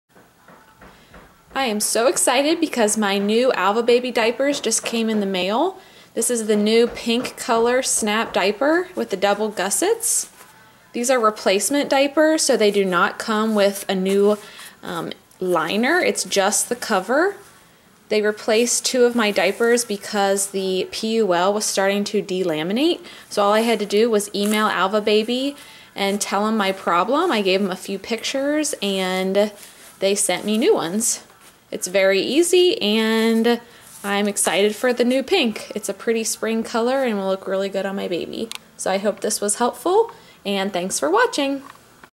New ALVA BABY Cloth Diaper!!
I just received my replacement Alva Baby double gusset diaper in the mail! Check out the new pink color :)